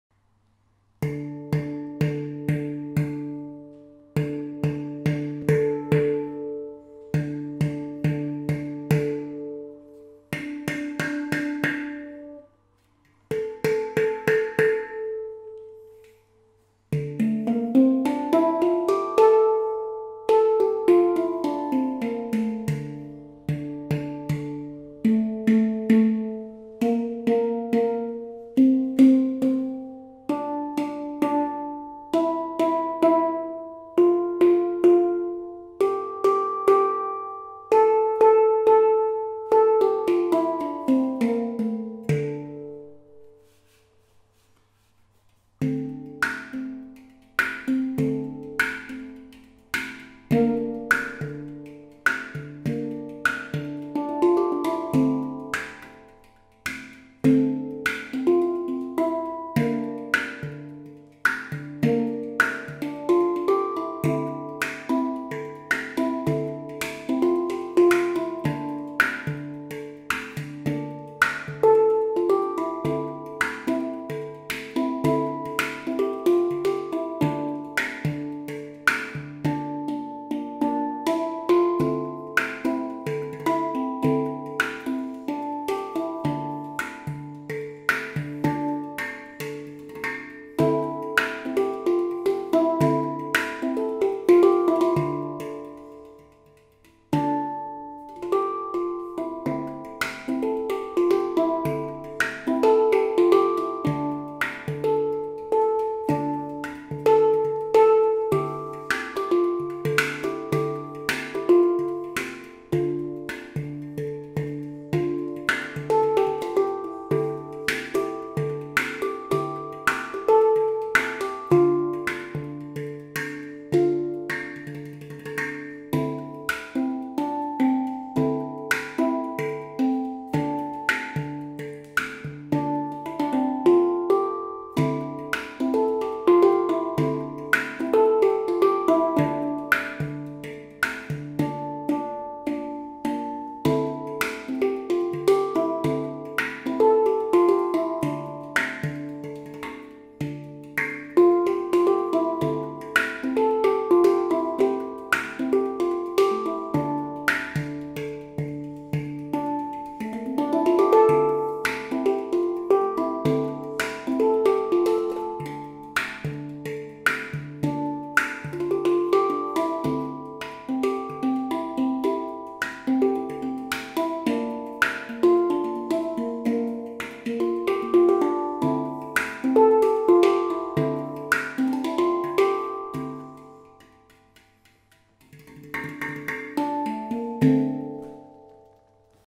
Haddock Instruments (HI-Pan) D Kurd 8
++ HEADPHONES RECOMMENDED ++

I'm very happy to present to you my latest creation. This is the sixth pan i've made and it's a D Kurd 8.
The first pan i've made with an inpex ding.
Feedback appreciated!